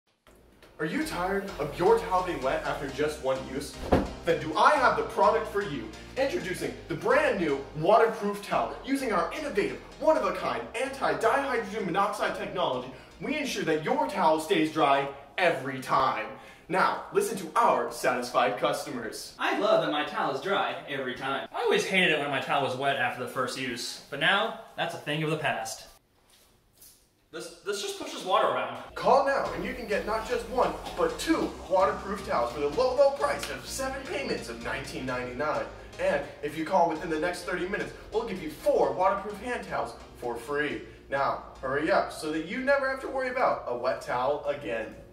The Water-Proof Towel!
Are you tired of your towel being wet after just one use? Try out the water-proof towel! The water-proof towel uses advanced technology that prevents it from ever getting wet. Never worry about your towel being wet again!